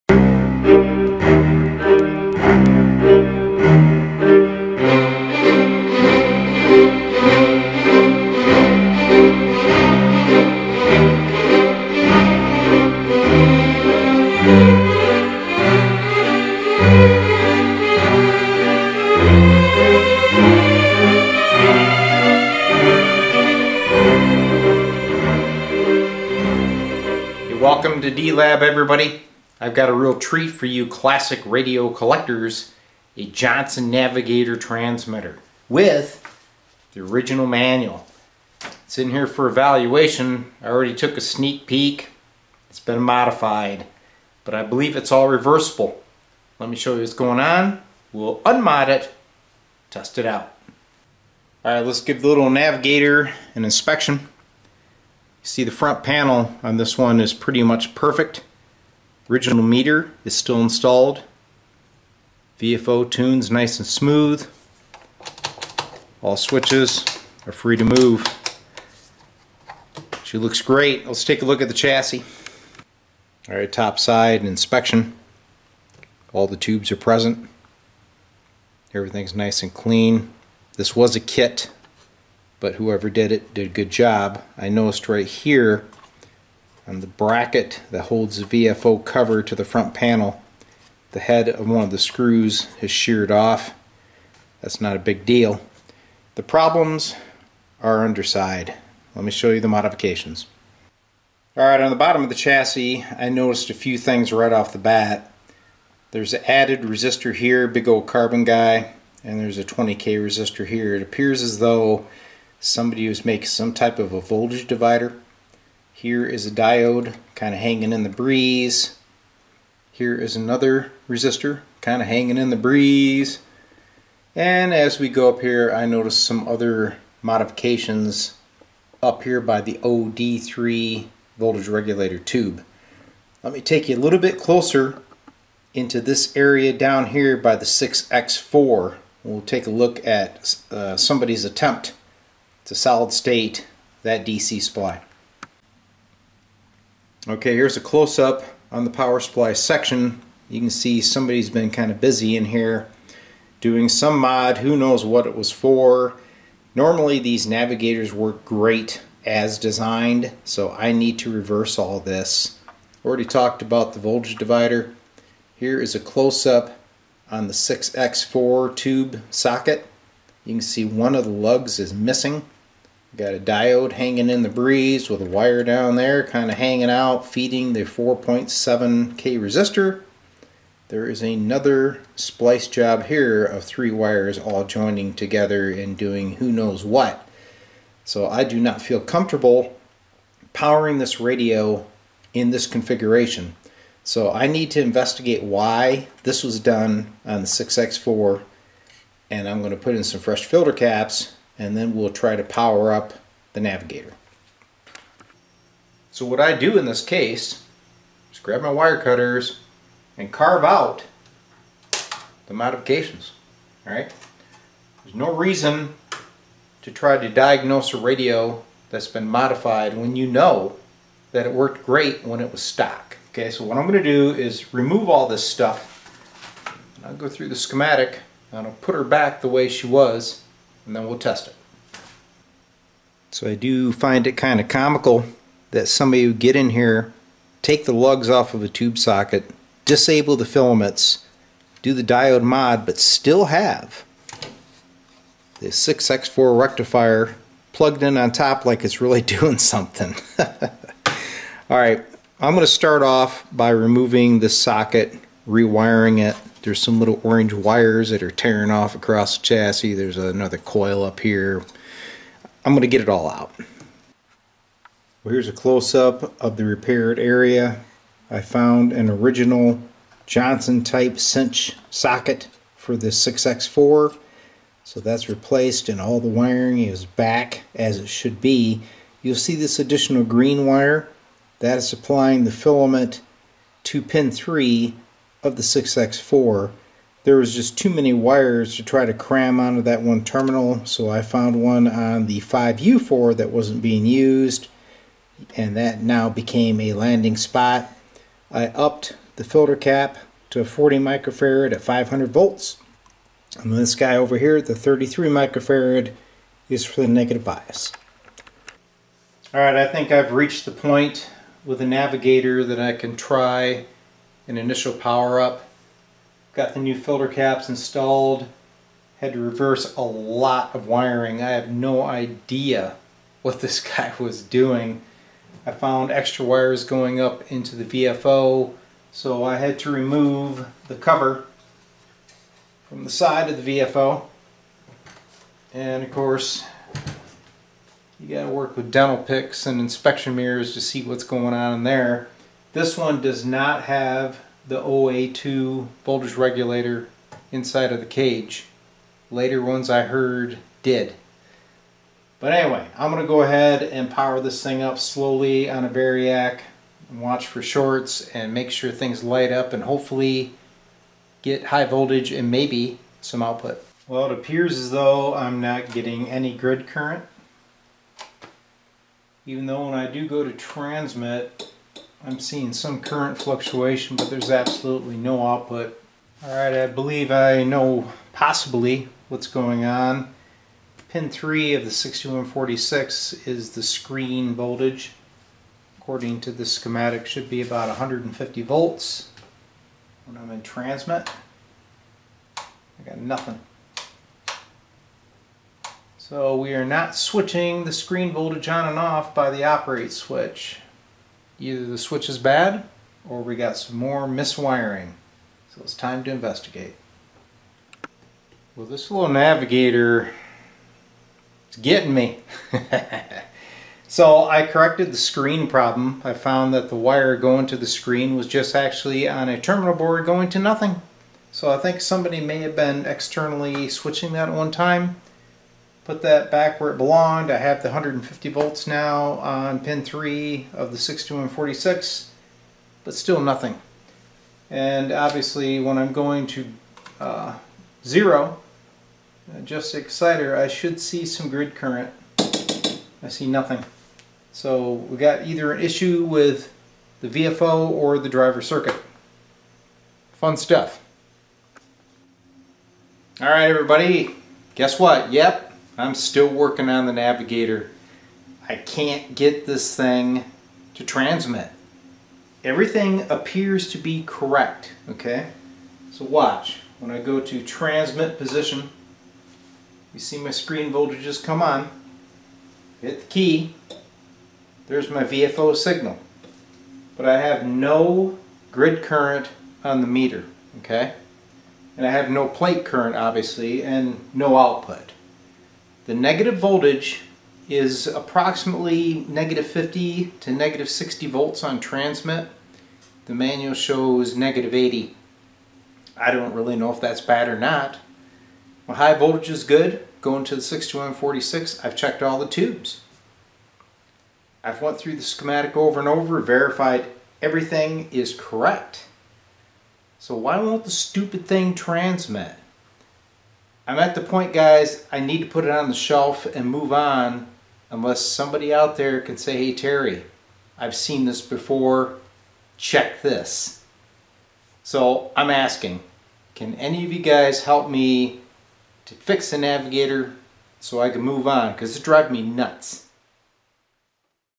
Johnson Navigator Ham transmitter repair D-Lab stumped need help no transmit difficult problem
Well, yep, I'm stumped. Normally performing an UN-Mod solves the problems. Not the case here. I do not know if this issue was always there, or if I induced it. Need to get this repair off the bench. If anyone has a good idea, have you seen this before? I'm all ears. Thank you

UPDATE: I found the issue. The VFO switch underneath is not in proper position. I manually adjusted it to match the band and boom! Transmits full power! Thanks everyone